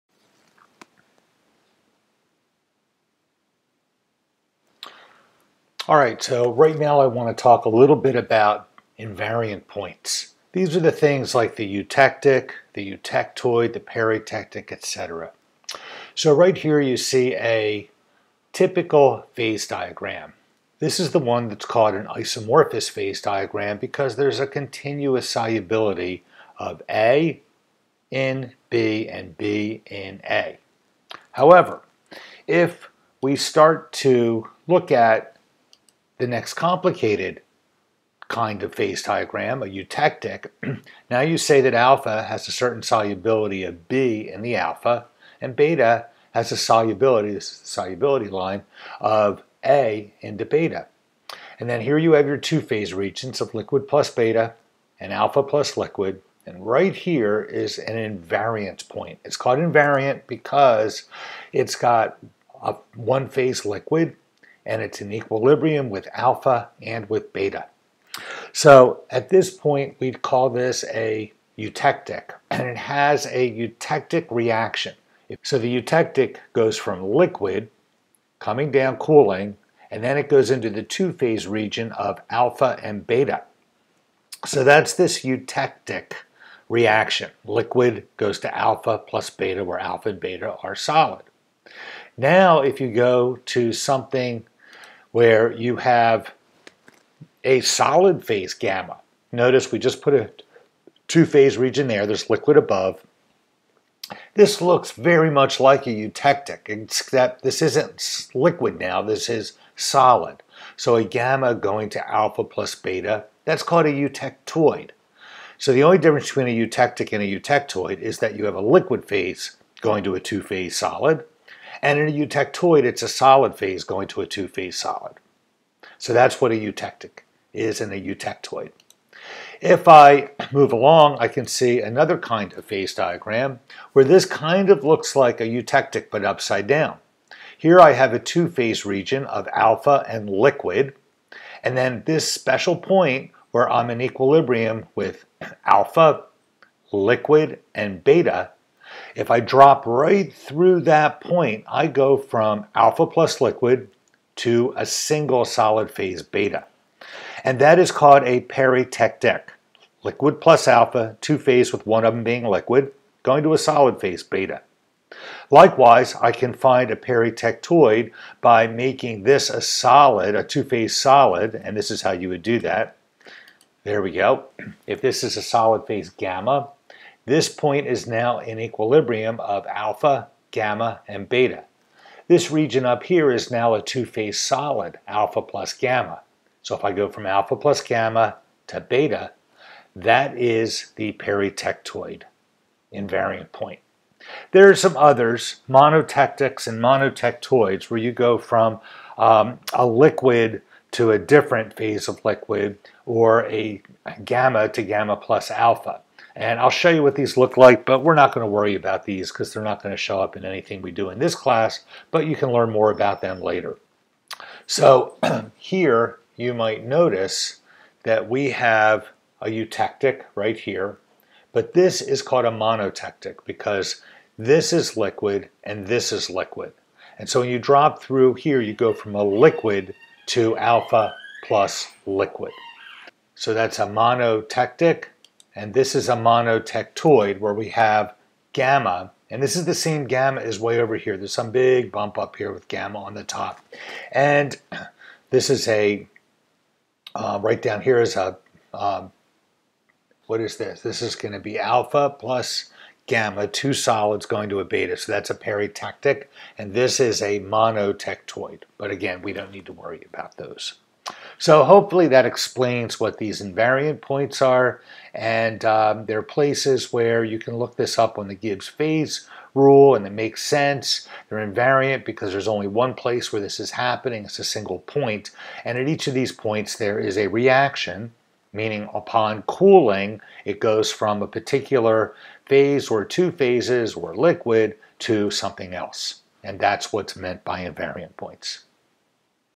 Invarient points
This short video describes invariant points such as a Eutectic, Eutectoid, Peritectic, etc.